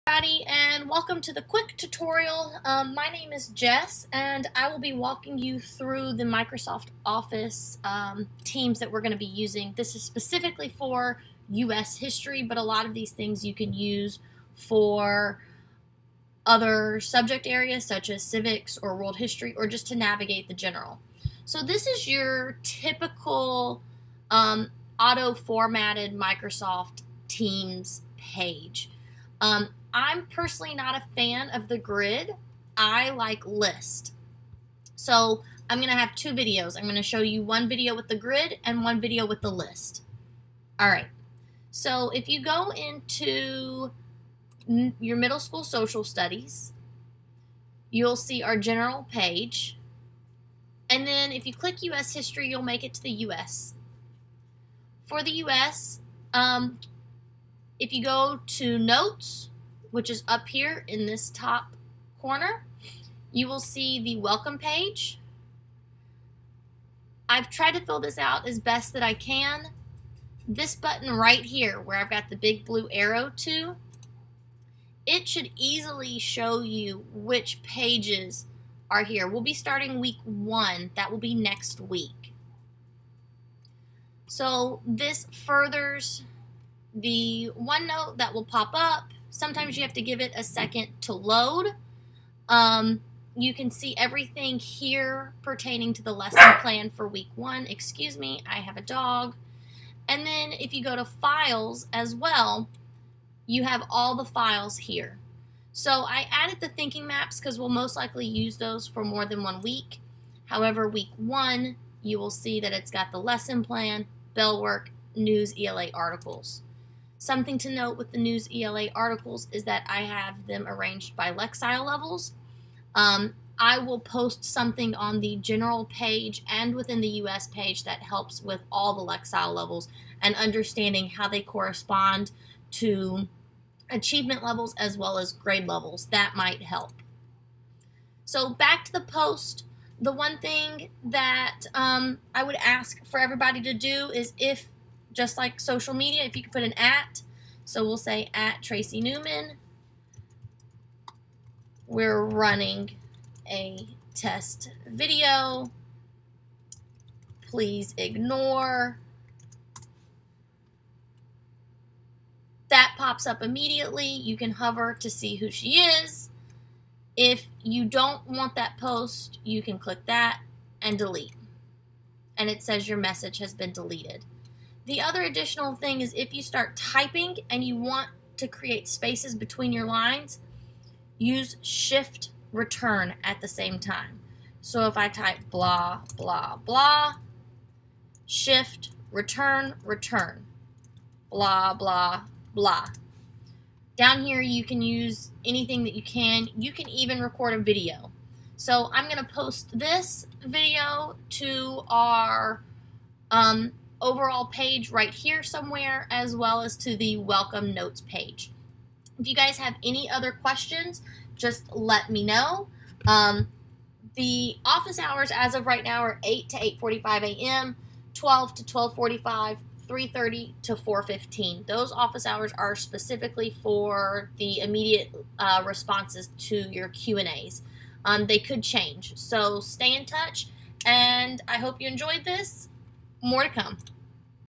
Microsoft Teams How To: Using Tile Display
Microsoft Teams how to change & navigate your display settings for Tile display.